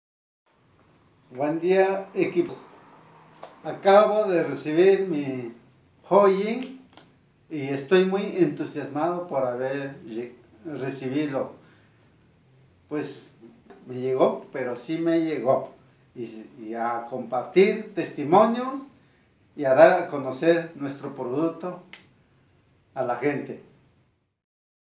TT1MM | Victorino Ventura - Me llegó el OGx Fenix
Desde Ciudad Juárez (México), Victorino Ventura y la señora Ana Santiago, nos envían este video con llagada del OGx Fenix (las malteadas como les llaman en México.

Con más de 70 años, Victorino Ventura es todo un ejemplo de valor, coraje y persistencia. ¿Por qué? Porque en lugar de ser un jubilado más, él elegió en 2014... ¡¡cambiar su vida gracias a ORGANO!!

¡¡Es fácil, es Simple... es ORGANO!!